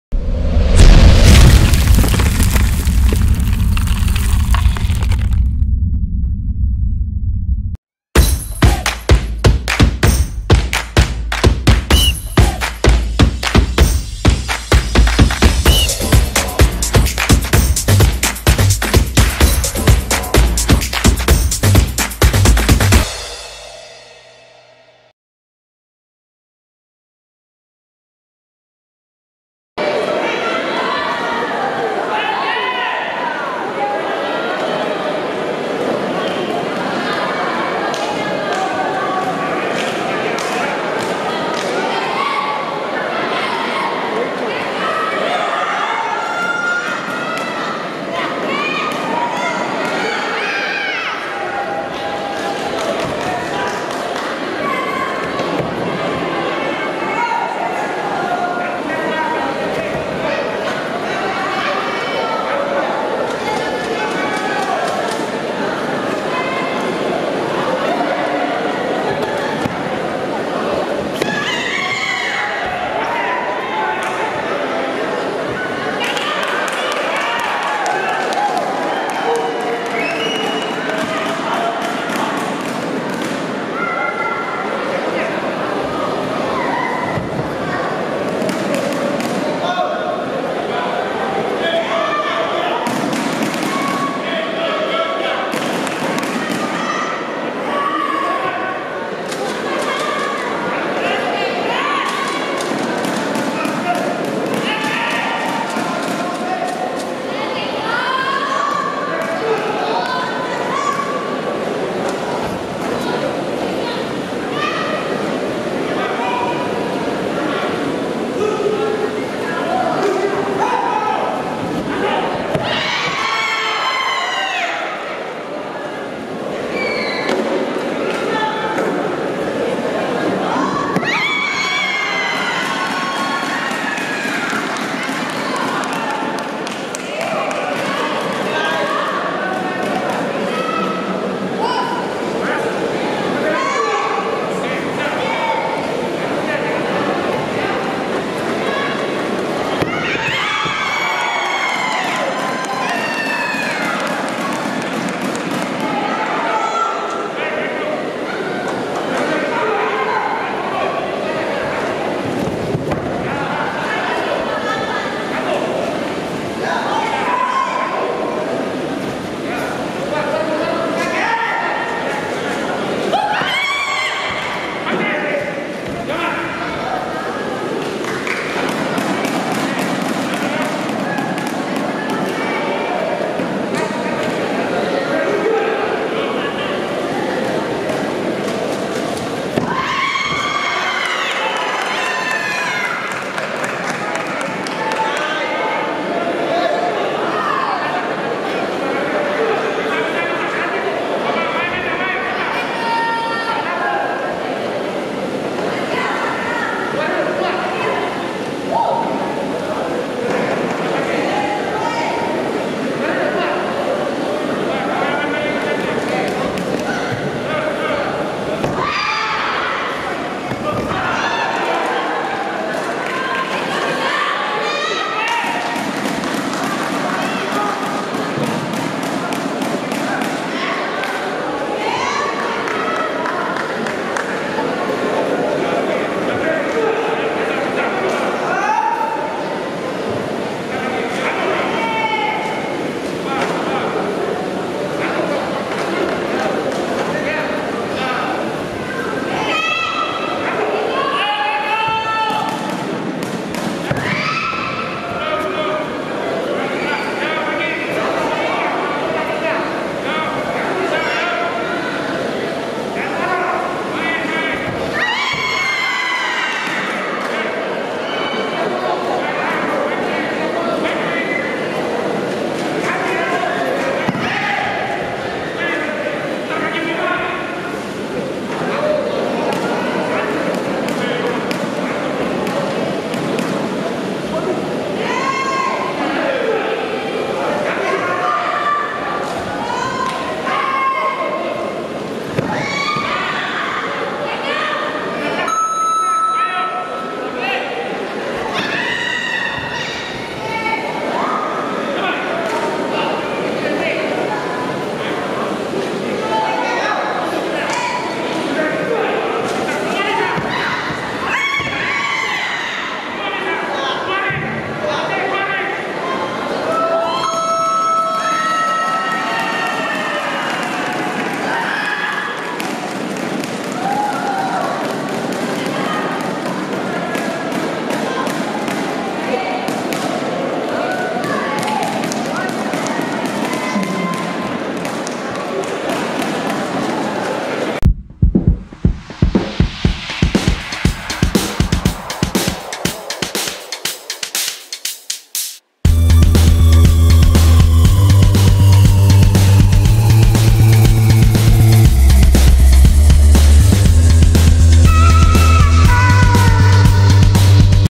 FINAL PON 2021 - Ceyco Georgia (DKI) Vs Wan Nurul (SUMBAR) - Kumite -68 Kg Putri
Send your MAIL for MAILTIME to
Ricky Muchtar

OSH